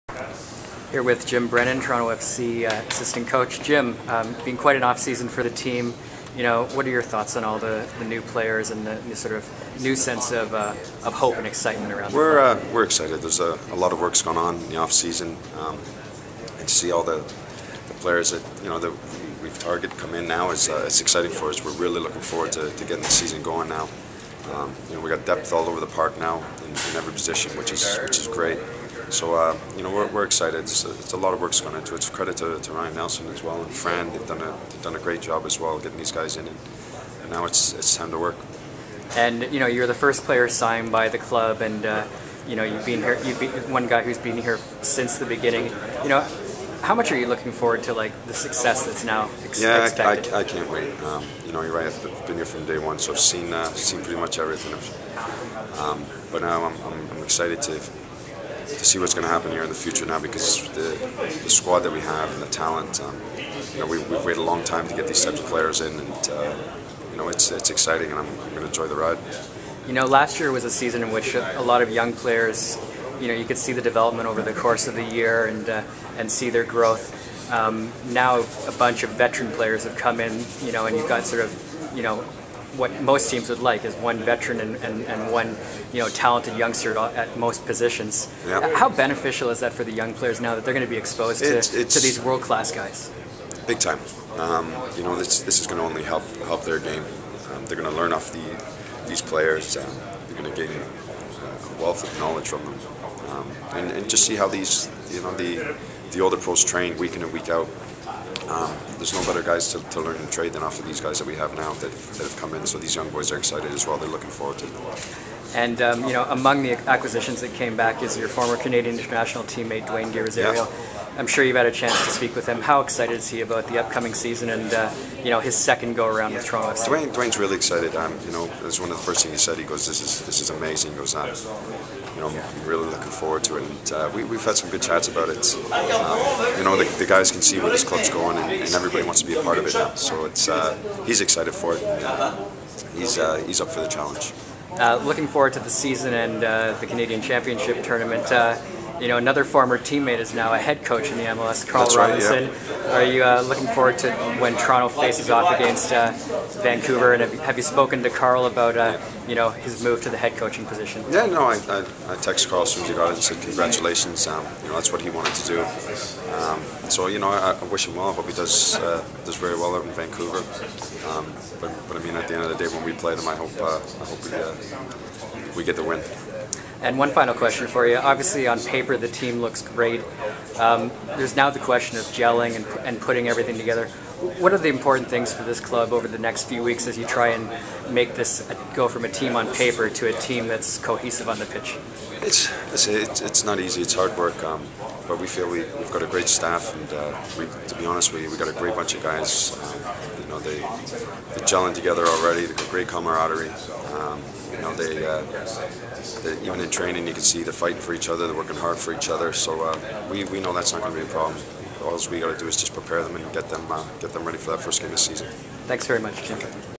Jim Brennan - Toronto FC preseason 2014
TFC Assistant Coach Jim Brennan speaks with RedNation during Toronto FC's preparations for the club's 2014 MLS campaign.